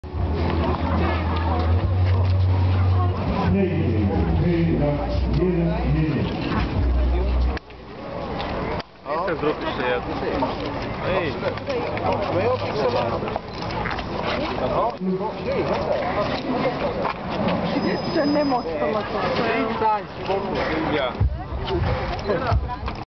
JOEY TEMPEST and MIC MICHAELI-WARSZAW BACKSTAGE
MEET EUROPE FANS AFTER SOUNDTEST IN WARSZAW-WIANKI 2010